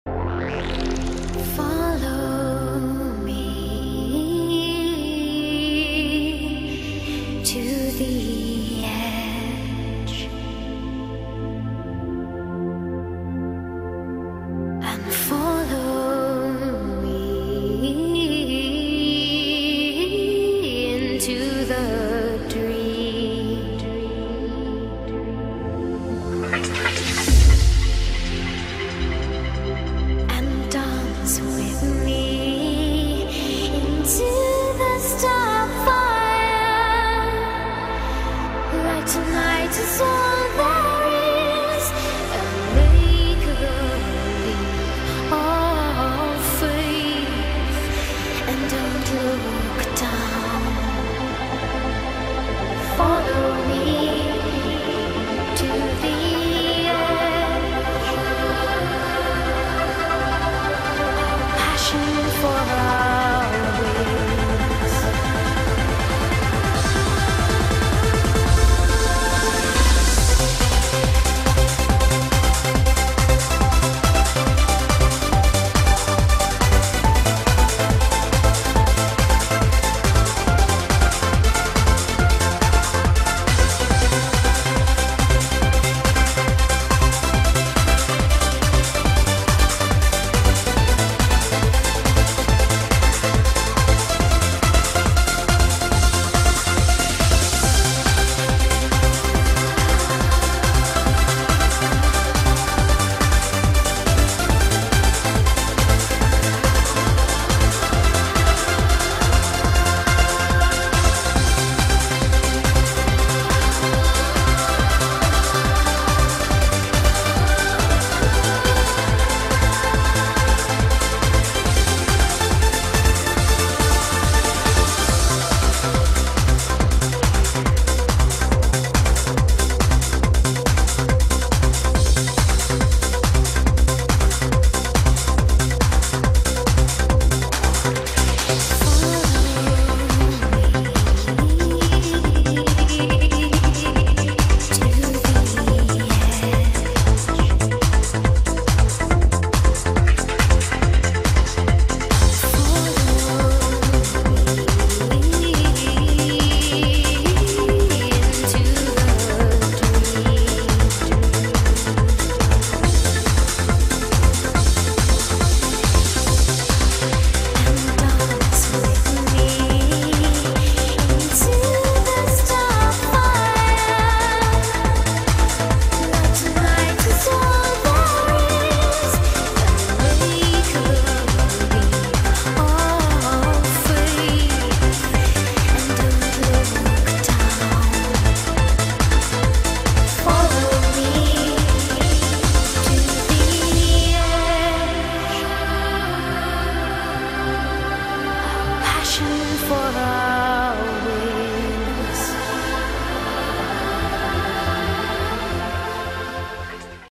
The King of Dakar - Marc Coma
This stuff made for celebrating Marc's second Dakar victory in 2009. Congrats Marc for winning the 2011 title!